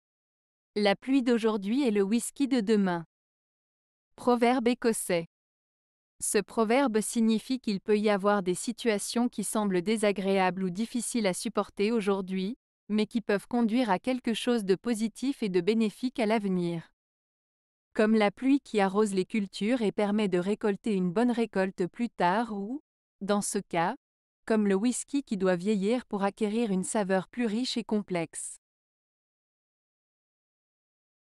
La pluie d'aujourd'hui est le whisky de demain. - Proverbe Ecossais
Ce proverbe signifie qu'il peut y avoir des situations qui semblent désagréables ou difficiles à supporter aujourd'hui, mais qui peuvent conduire à quelque chose de positif et de bénéfique à l'avenir. Comme la pluie qui arrose les cultures et permet de récolter une bonne récolte plus tard ou, dans ce cas, comme le whisky qui doit vieillir pour acquérir une saveur plus riche et complexe.